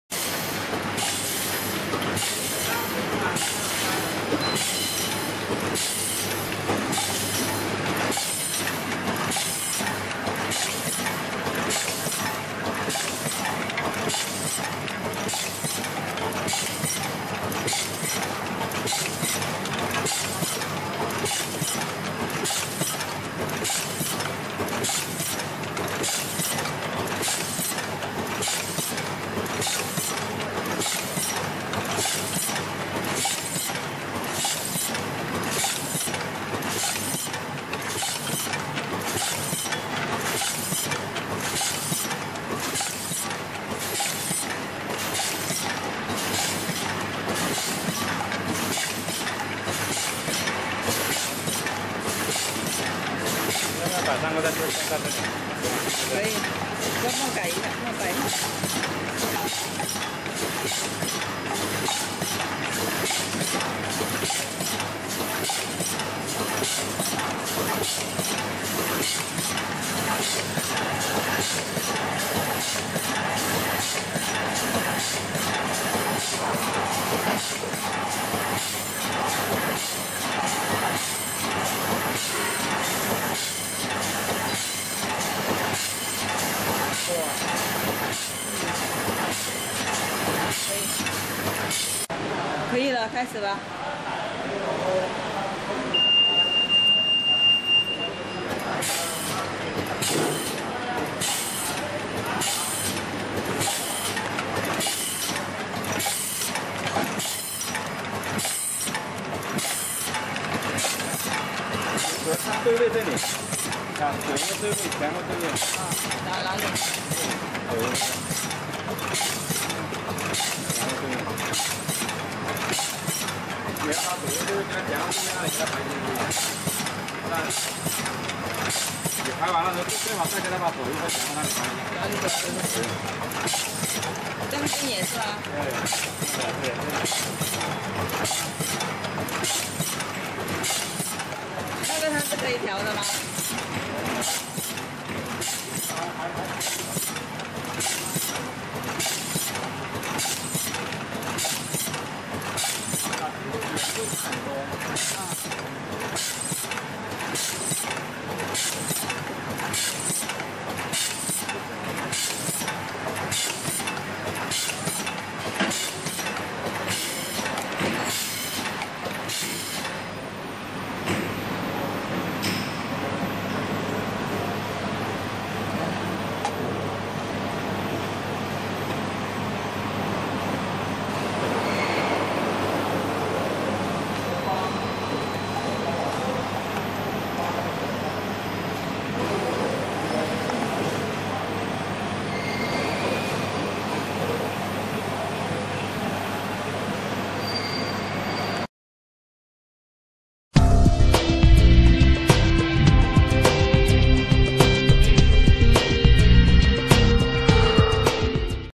Automatic Screen Printing Machine For Cosmetic Tube
Automatic Screen Printing Machine For Cosmetic Tub
Automatic Screen Printing Machine for Cosmetic Plastic Tube
This machine is special for 1 color printing of soft tube, bottle cap and which is similar with the special plastics container. The Max printing speed is 4000pcs/ hour. This machine combine with: advanced PLC、touch operation screen and display control system、fiber digital servo system (the second printing),automatic feeding and unloading conveyer system、flame treatment system  , high/low speed mechanical sync transport system, automatic alignment function, and “not work piece not printing” sensor, diagonal mechanical printing fix system.

Feature:
1.the machine is controlled by micro-computer which is stability and reliability, with a clear bright blue screen so that the machine is operated easily and maintained convenient.
2.international famous pneumatic units such as brands of Festo and so on which are selected for the machine, the speed can be adjusted independently.
3.the machine is designed for round objects, it applies to plastic soft bottle, plastic bottle, cosmetic etc.
4.the machine is convenient to select movable printing head for round printing Item.